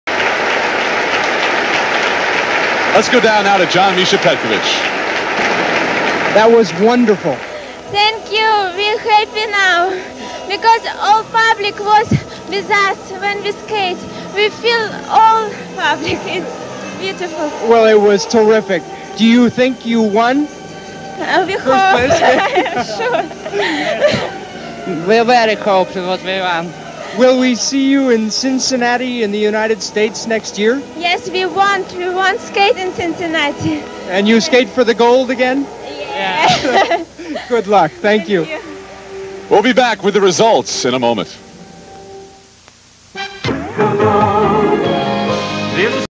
Figure Skating - 1986 - John Petkevich Interviews World Ice Dance Champs USSR Bestemianova + Bukin
has the largest and most diverse vintage FIGURE SKATING video library online